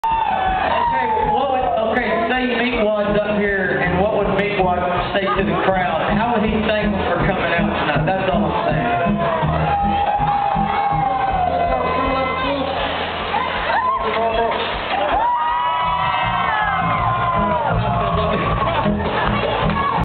Damian as Meatwad
Damian Green, Aaron Watson's fiddle player, is a big fan of Aqua Teen Hunger Force. He graced the Brewster Street audience with his impression of Meatwad on 10.15.09 :]